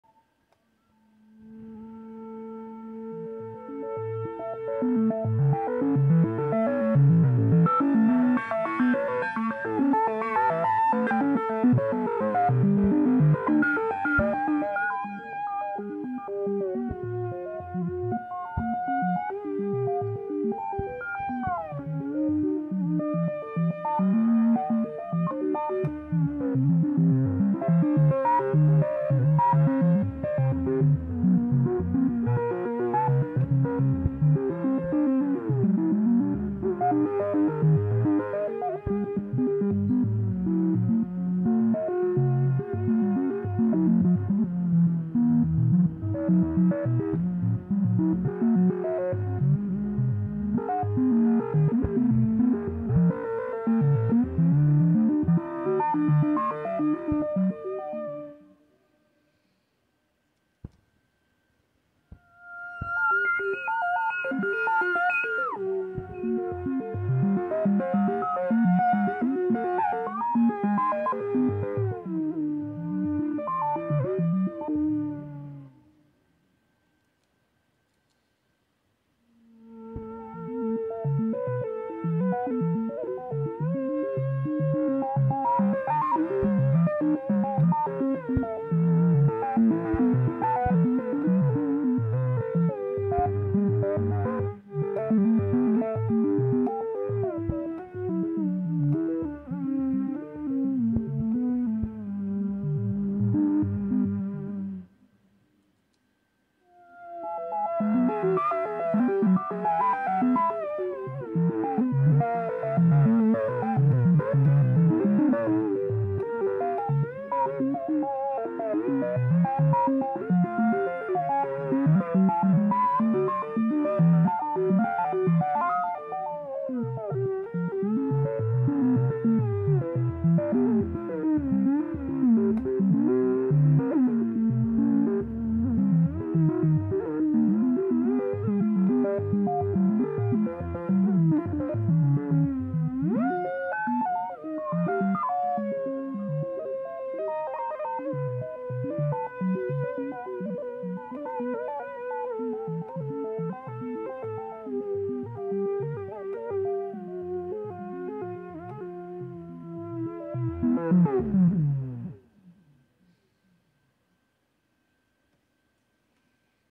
trillActionsByTheFootSwitch
Must be switching by the left foot because of the pitch drift occur from the body moving on the right side.